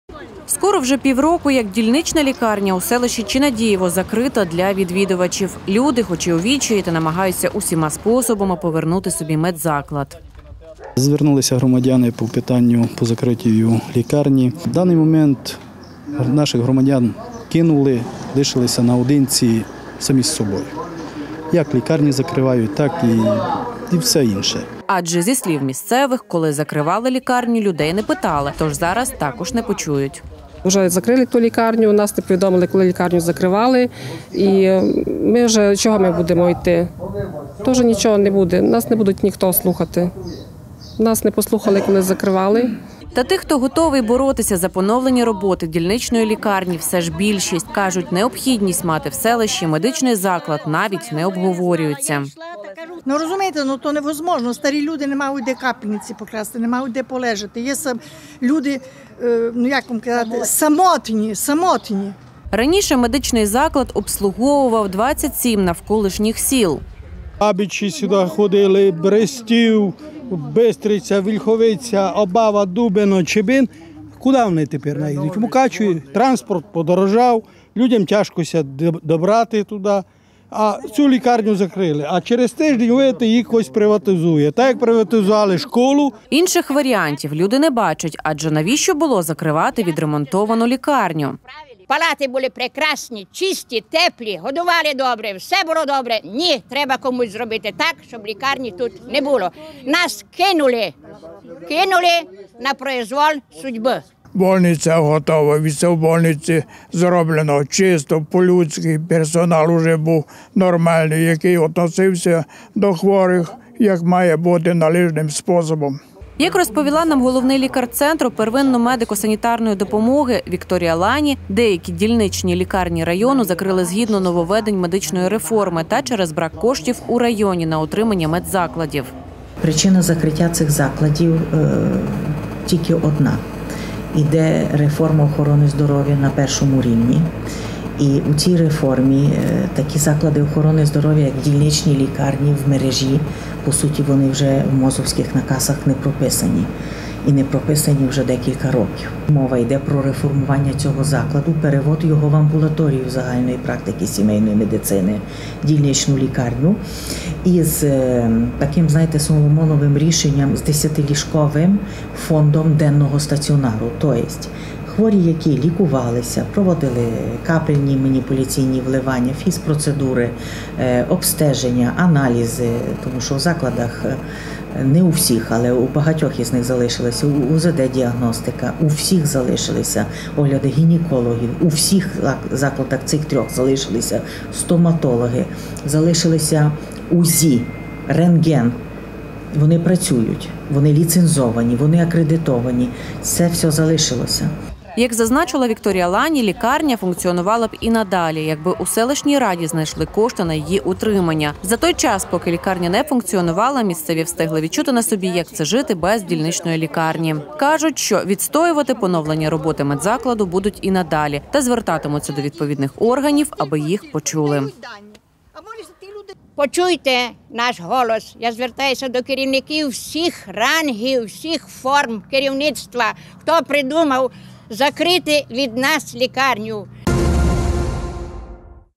Без лікарні
Скоро вже півроку, як дільнична лікарня в смт. Чинадієво Мукачівського району закрита для відвідувачів. Люди хоч у відчаї, та намагаються усіма способами повернути собі медзаклад. Зі слів місцевих жителів, коли закривали лікарню – людей не питали, і зараз також не почують. Вважають, що медичний заклад, який колись обслуговував 27 навколишніх сіл, закрили, щоб хтось міг його приватизувати. Та тих, хто готовий боротися за поновлення роботи дільничної лікарні в смт. Чинадієво, все ж більшість. Кажуть, необхідність мати в селищі медичний заклад навіть не обговорюється.